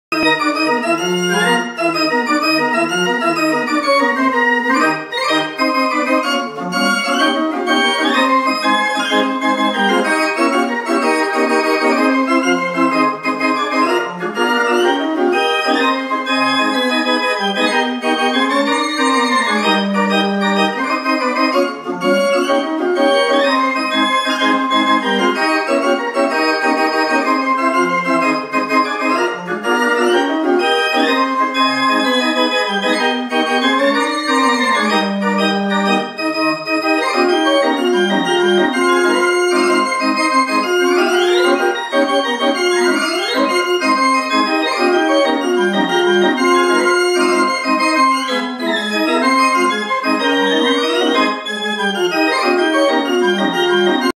86 Note Organ Playing.MOV
This is an 86 note self contained pipe organ that plays from an internal midi player using an SD card.  The pipes are from an old church pipe organ, while the chest and internals are new.